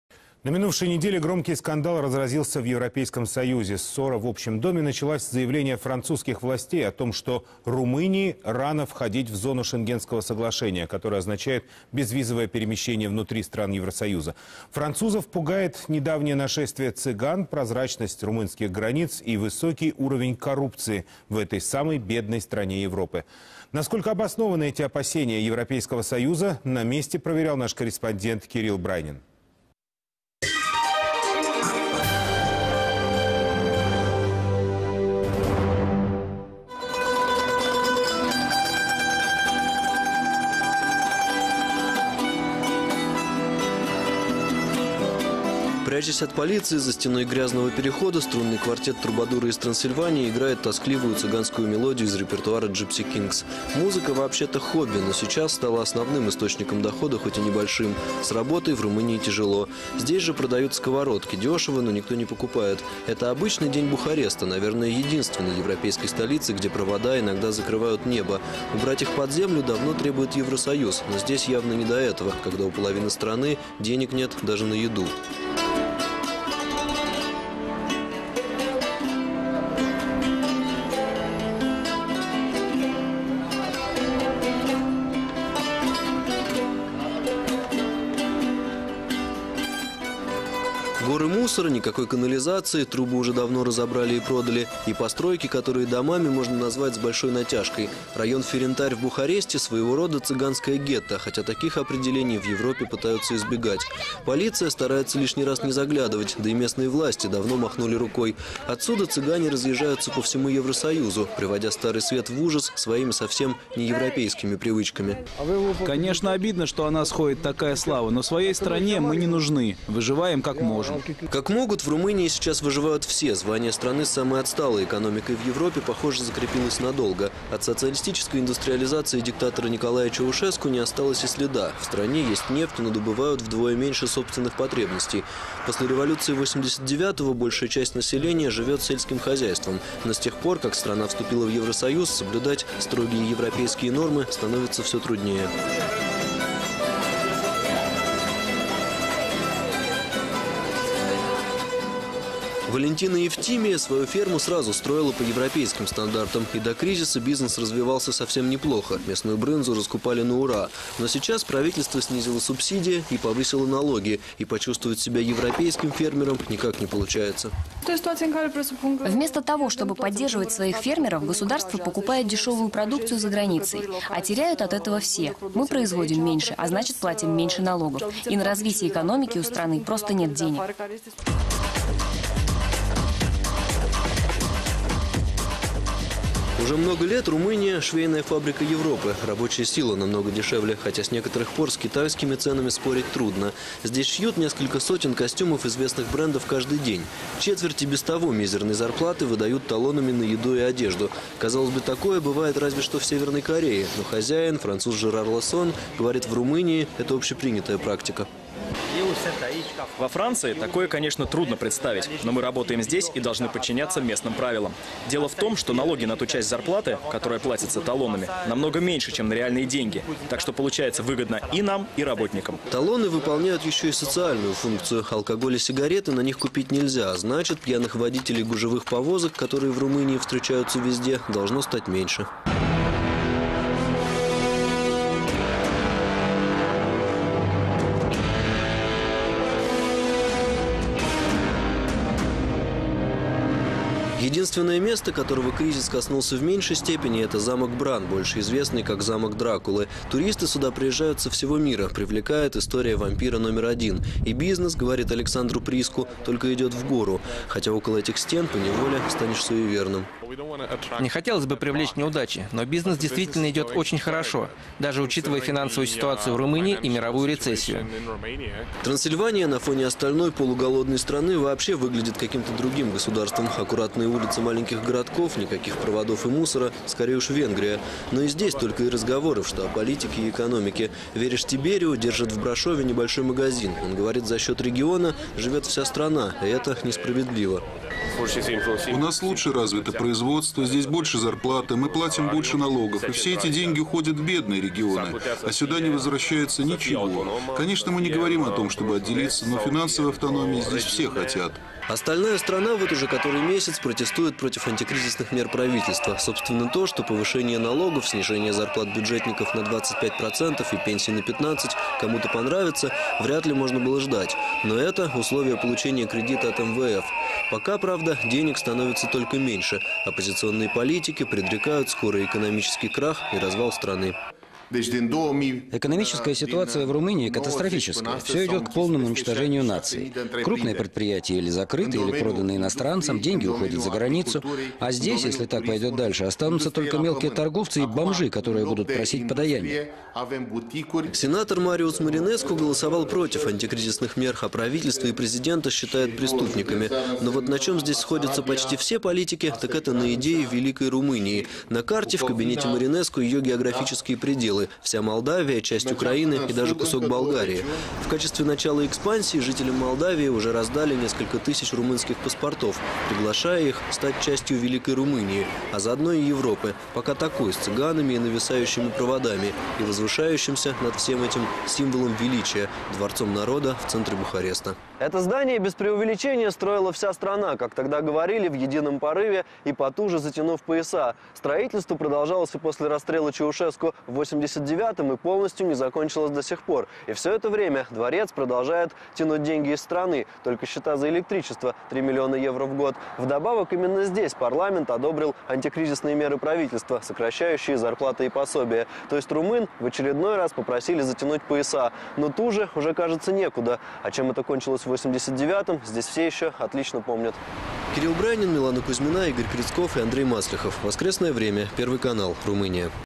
Televiziunea Oficială din Rusia: ”România, cea mai săracă din Uniunea Europeană”
Ce au filmat jurnaliștii ruși în România, în București? Populație romă, sărăcie, Casa Poporului și... cabluri.